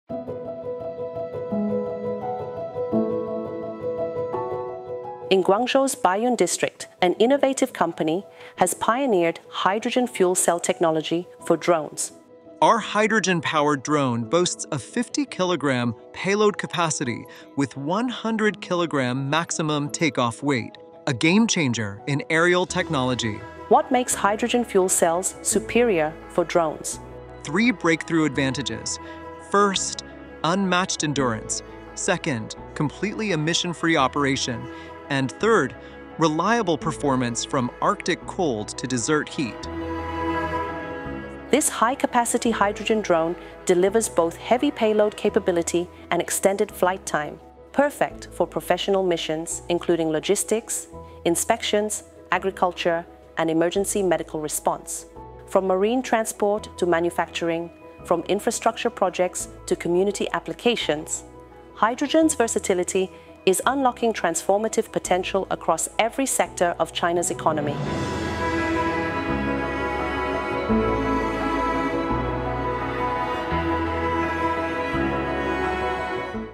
[Carbon Revolution] Hydrogen Soars: The Drone Revolution Taking Flight
Guangzhou's hydrogen drones redefine aerial capability: 50kg payloads, extreme-weather reliability, and zero emissions. Another leap in China’s hydrogen revolution, now taking to the skies.